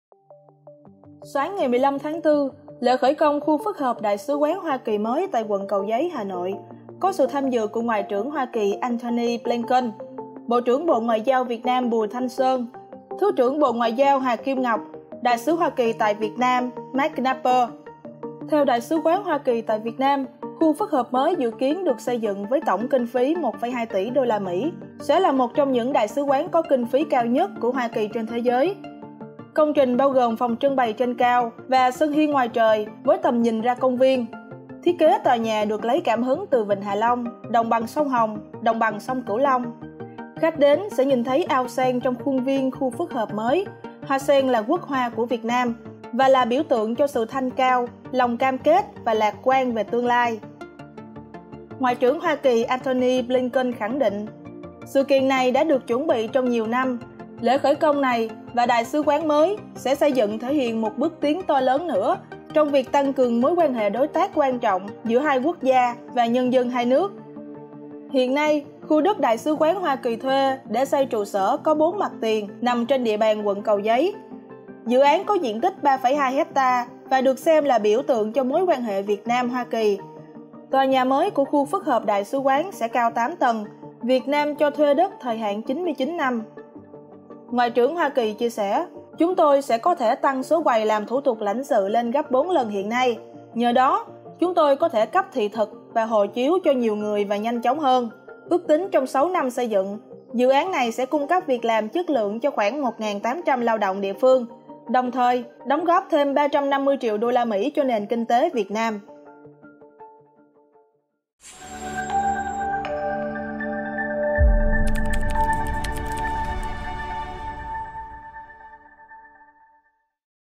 Khu phức hợp Đại sứ quán Hoa Kỳ xây mới tại Hà Nội với kinh phí 1,2 tỷ USD
Theo Đại sứ quán Hoa Kỳ tại Việt Nam Marc Knapper, khu phức hợp mới dự kiến được xây dựng với tổng kinh phí 1,2 tỷ USD, sẽ là một trong những đại sứ quán có kinh phí cao nhất của Hoa Kỳ trên thế giới.